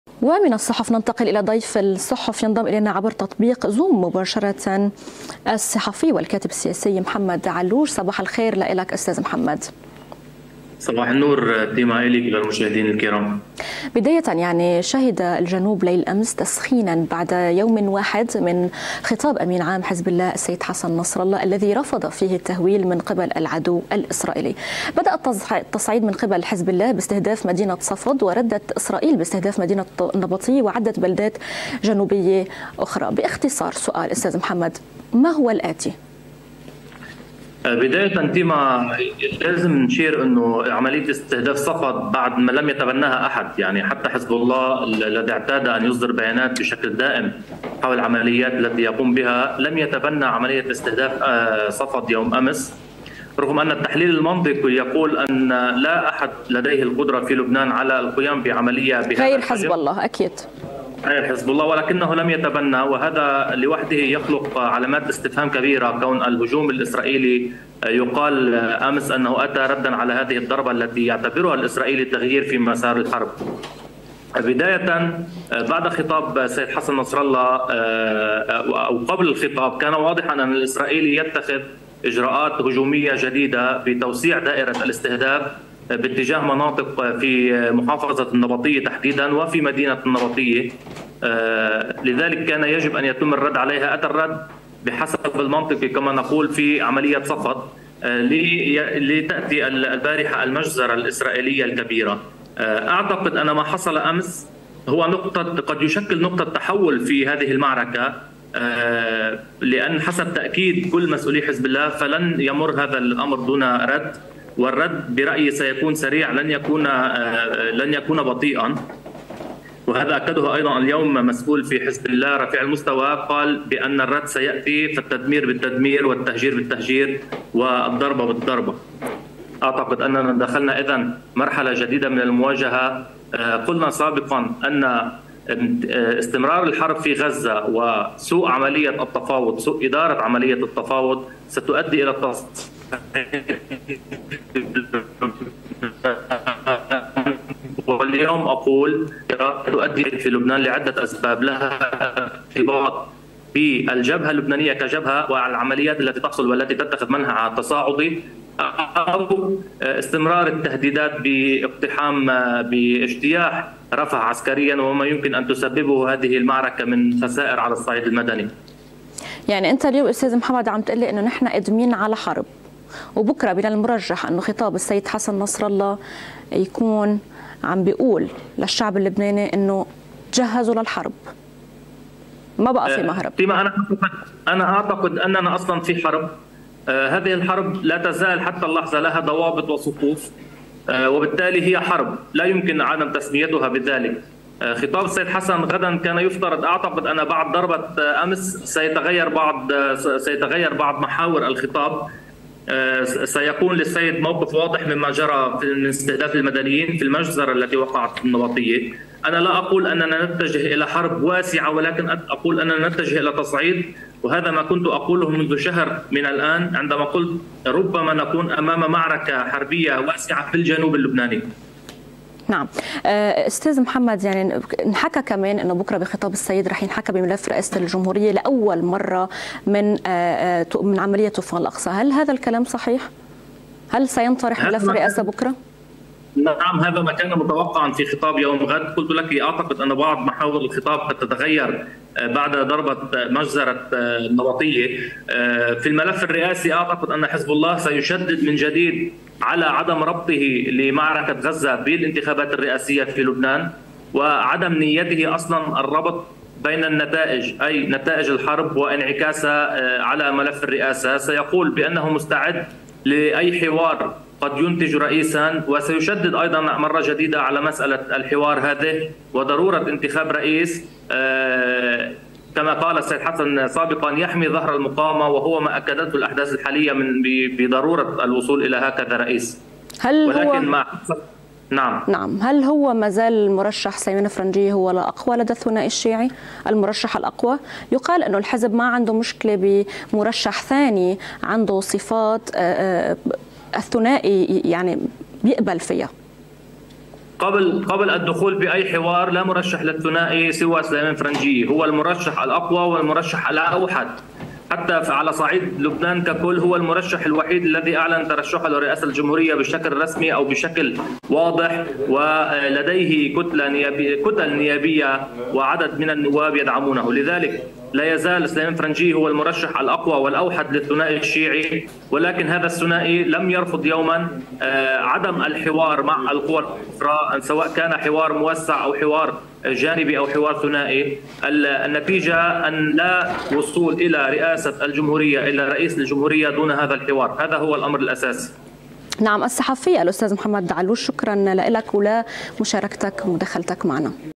"اجراءات اسرائيلية" تثير الجدل وصحافي يسرب: متجهون الى التصعيد
قال الكاتب السياسي محمد علوش فيء اتصال مع قناة الجديد :  من الواضح ان الاسرائيلي يتخذ إجراءات جديدة بتوسيع نطاق الحرب وما حصل أمس في النبطية يشكل نقطة تحول جديدة